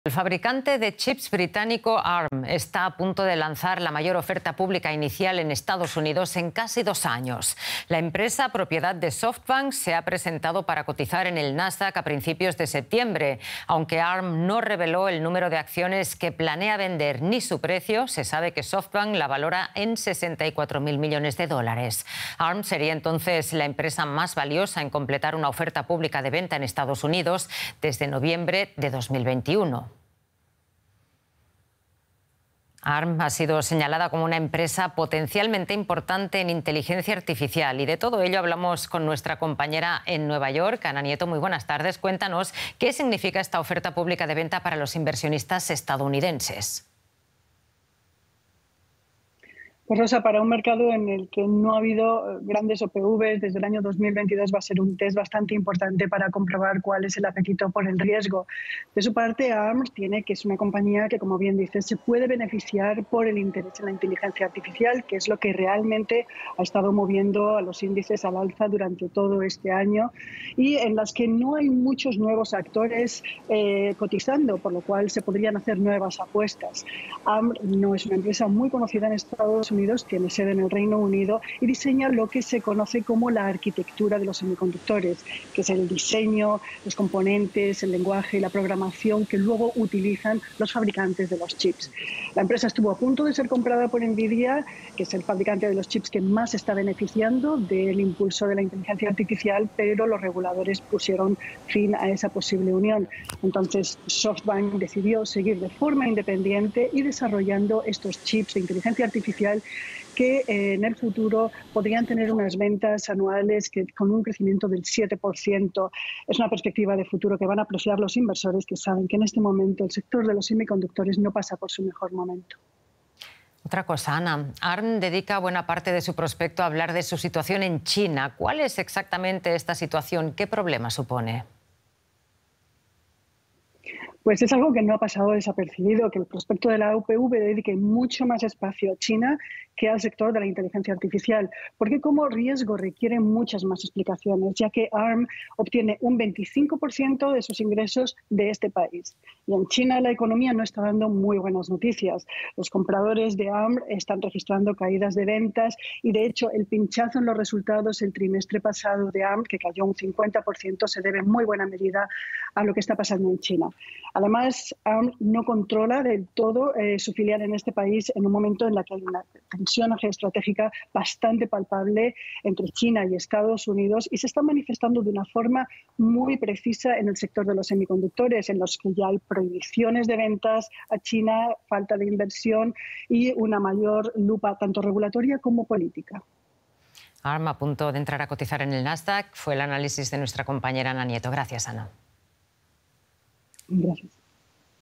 Empresa británica de microchips será la más valiosa en salir en bolsa en dos años en EE. UU.
ARM, la firma británica de chips, se dispone a lanzar la mayor oferta pública en casi 2 años en EE. UU., optando por cotizar en Nasdaq en septiembre. La empresa, valorada en 64 mil millones de dólares por Softbank, se convertiría en la más valiosa en completar una oferta pública desde noviembre de 2021.